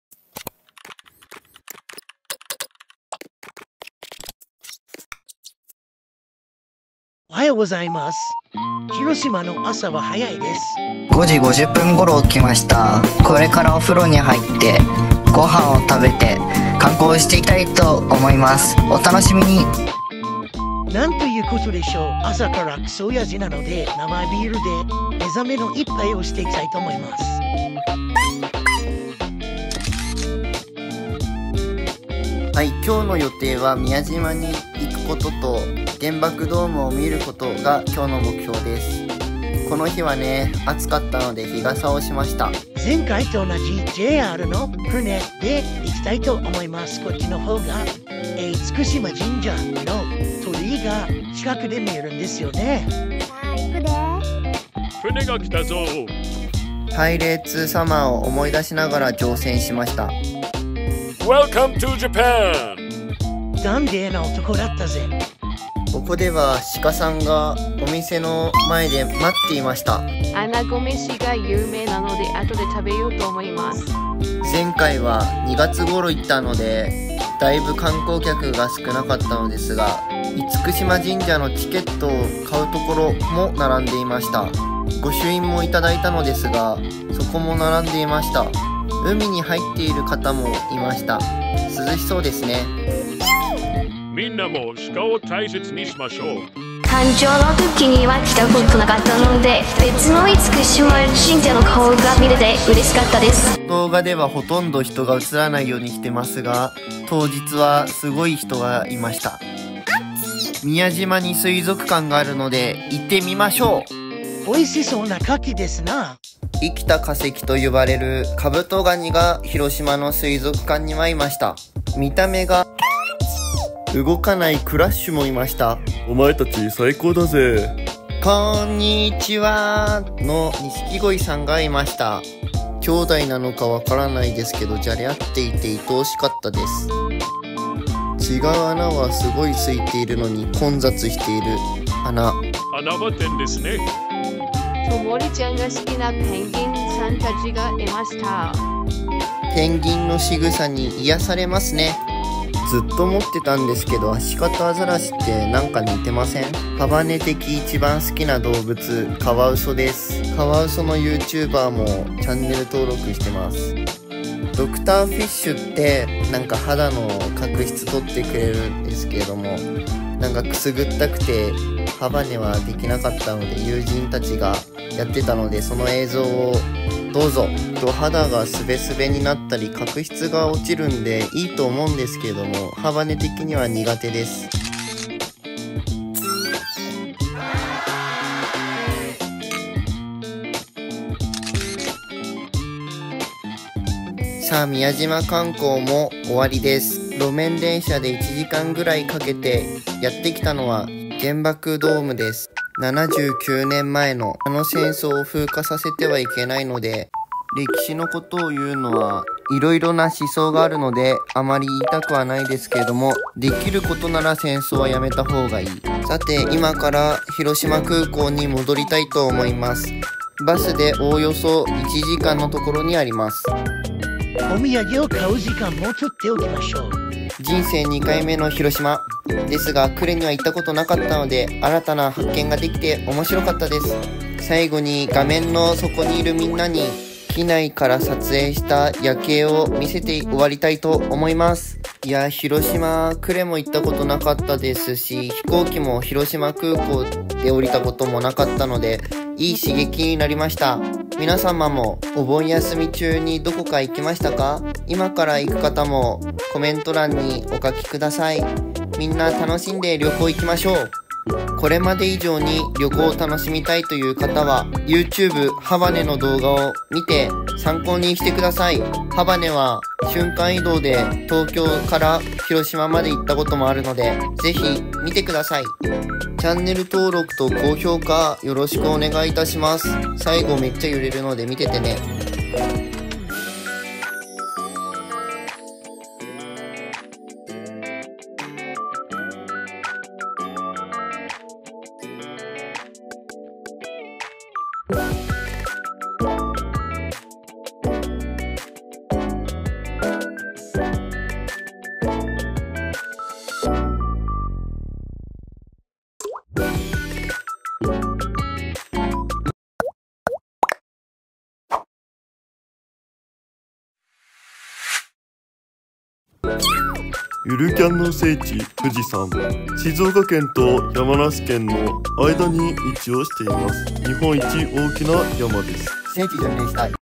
【広島観光】宮島のおすすめ観光スポット紹介
最近は台風が多くなりましたね。台風が多いと洪水や災害が起こるかもなんで避難できる場所の確認とか必要です。

今回は、物や動物に音声をつけてみました😊某キャンプアニメのリスペクトをしながら行いました。
甲高い声は男なんで厳しいって
この動画が将来役に立つ日が来ることをひそかに願っています。

動画編集とか動画を観てもらって誰かを支えられるような仕事がしたい。映像系のお仕事

そんで再生数が伸びればいいんだよね（理想）